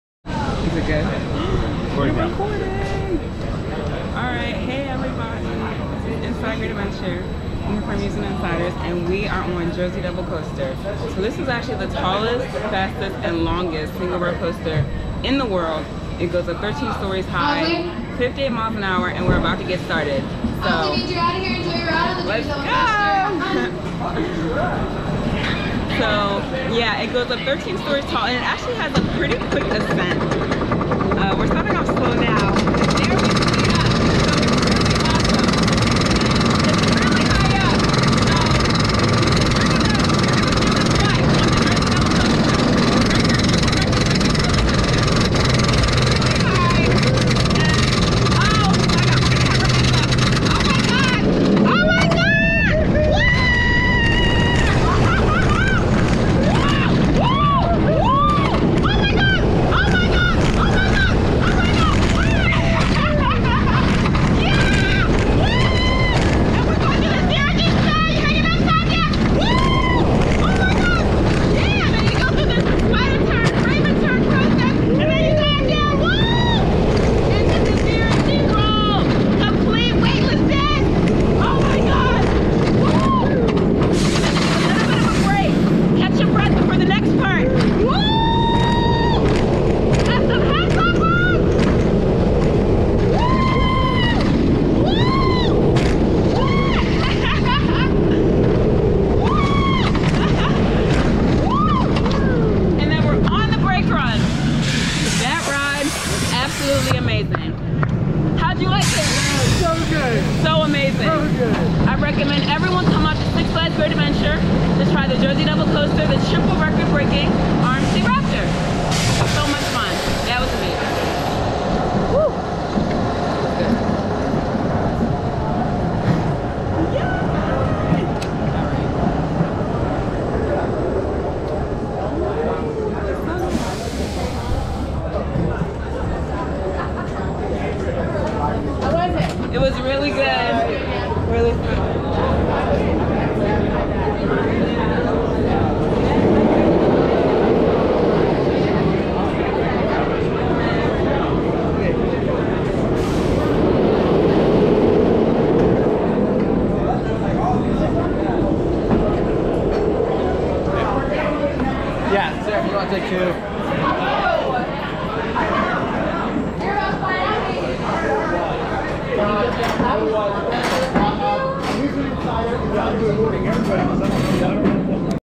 Six Flags Jersey Devil Rider Reaction
InsideGADV from Instagram takes a ride on Jersey Devil! Have you had an opportunity to ride?